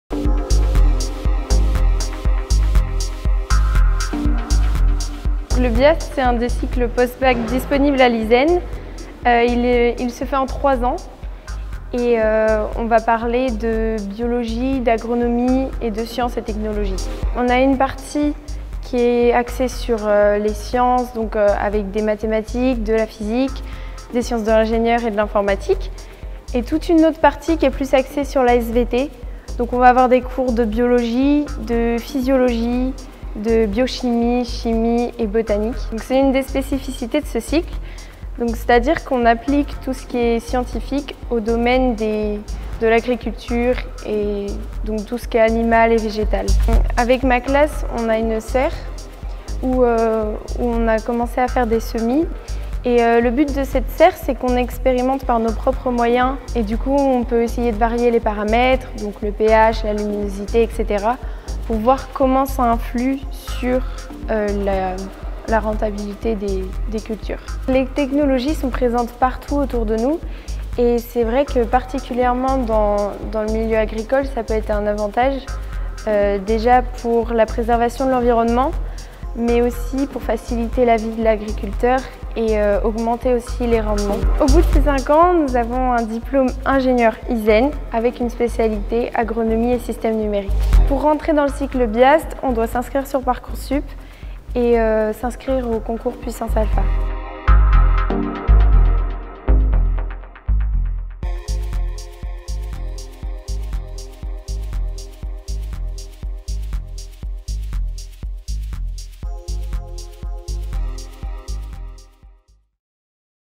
Devenir Ingénieur : Biologie, Agronomie, Sciences et Technologies (BIAST)
Lilwenn nous explique pourquoi elle a choisi le Cycle Biologie, Agronomie, Sciences et Technologies (BIAST) pour suivre ses études à l'ISEN.

Le cycle "Biologie, Agronomie, Sciences et Technologies" (BIAST) est l'un des cycles d'entrée dans la formation d'ingénieur post-bac de l'ISEN (Brest). Ce cycle dure 3 ans.
À l’issue du BIAST, les étudiants peuvent se diriger vers tous les domaines professionnels proposés en années 4 et 5 à l'ISEN - sans concours - notamment le domaine professionnel « Agriculture et Numérique » qui permet d’obtenir un diplôme d’ingénieur de spécialité de l’ISEN Yncréa Ouest.

Les étudiants qui intègrent le BIAST sont des étudiants ayant un attrait particulier pour la biologie, l'agronomie, les sciences de la vie et de la terre, mais également pour les technologies numériques. Ce sont des élèves souhaitant disposer d'une formation théorique solide tout en suivant un cursus très tourné vers la pratique et la réalité des entreprises.

Le cycle "Biologie, Agronomie, Sciences et Technologies" est proposé sur le site de l'ISEN de Brest

Le BIAST est ouvert aux Terminales et accessible sur ParcourSup (Concours Puissance Alpha)

Transcription de la vidéo :

Le BIAST c'est un des cycles post-Bac disponible à l'ISEN. Il se fait en trois ans et on va parler de Biologie d'Agronomie et de Sciences et Technologies. On a une partie qui est axée sur les sciences donc avec des mathématiques, de la physique, des sciences de l'ingénieur et de l'informatique, et toute une autre partie qui est plus axée sur la SVT, donc on va avoir des cours de biologie, de physiologie, de biochimie chimie et botanique.

C'est une des spécificités de ce cycle, donc c'est à dire qu'on applique tout ce qui est scientifique au domaine de l'agriculture et donc tout ce qui est animal et végétal.

Avec ma classe, on a une serre où on a commencé à faire des semis et le but de cette serre c'est qu'on expérimente par nos propres moyens et du coup on peut essayer de varier les paramètres donc le pH, la luminosité... pour voir comment ça influe sur la rentabilité des cultures.

Les technologies sont présentes partout autour de nous et c'est vrai que particulièrement dans le milieu agricole ça peut être un avantage, déjà pour la préservation de l'environnement mais aussi pour faciliter la vie de l'agriculteur et augmenter les rendements. Au bout de cinq ans, nous avons un diplôme Ingénieurs ISEN avec une spécialité "Agronomie et Systèmes Numériques".

Pour rentrer dans le cycle BIAST on doit s'inscrire sur parcoursup et s'inscrire au concours Puissance Alpha.